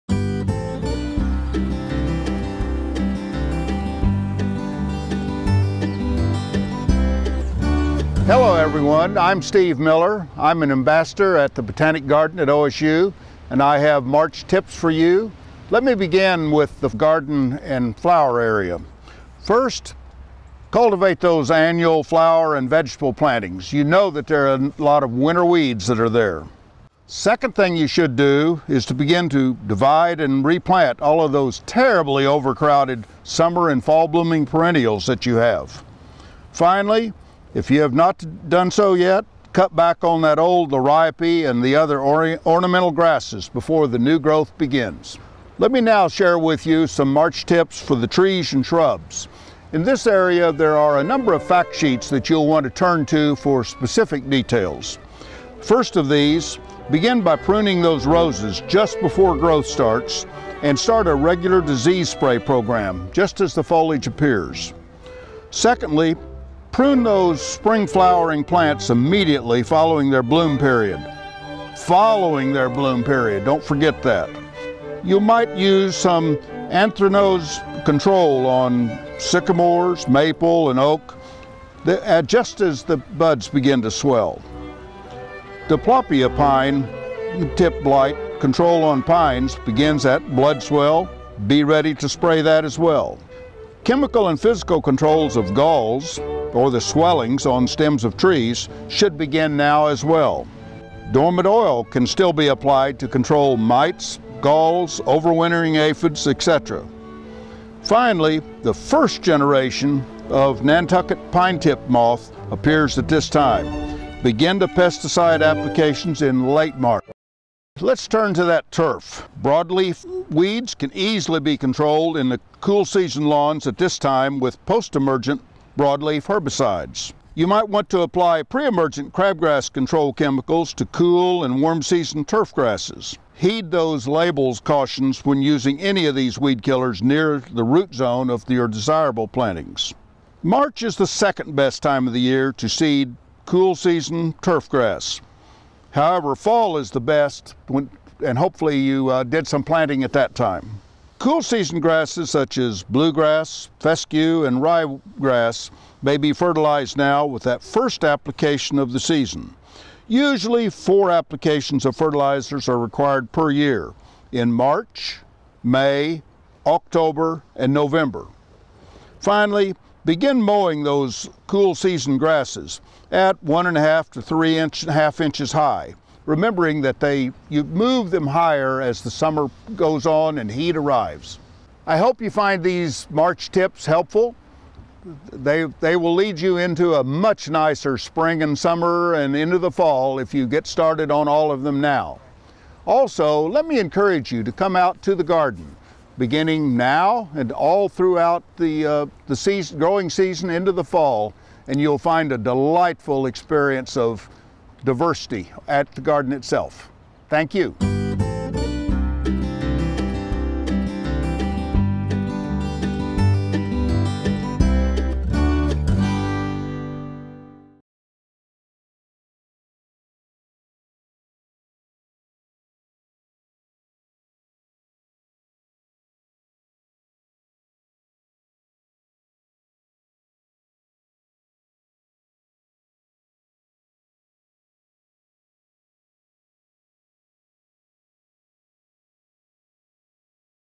Horticulture Tips for March
(02/22/14)-Botanic Garden at OSU Ambassador Steve Miller shares with us some horticulture

EPP 7634 Anthracnose and Other Common Leaf Diseases of Deciduous Shade Trees